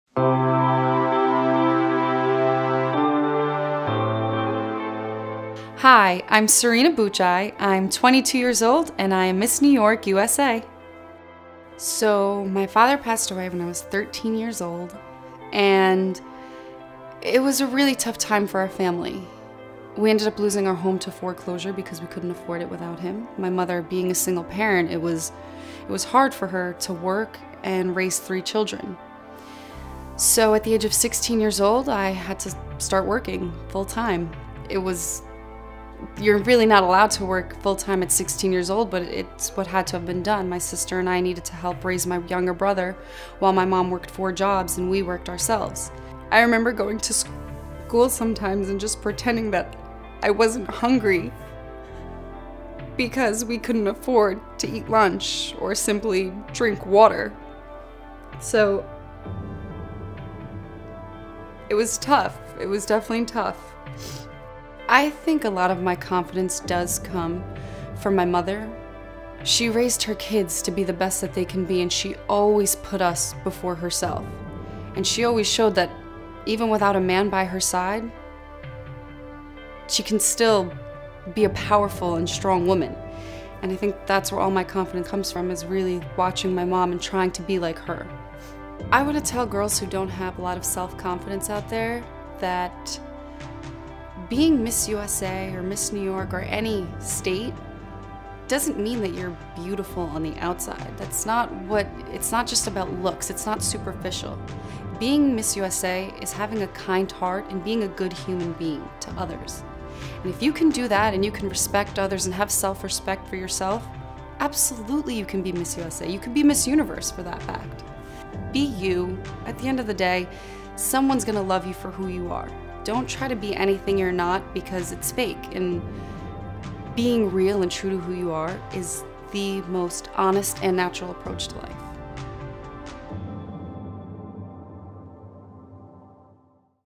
New York: Serena Bucaj - Road to Vegas
2016 Miss USA Contestant: Miss New York USA
"...Miss USA is about having a kind heart and being a good human being to others." Hear more about Miss New York USA Serena Bucaj's journey and ‪#‎RoadToVegas‬,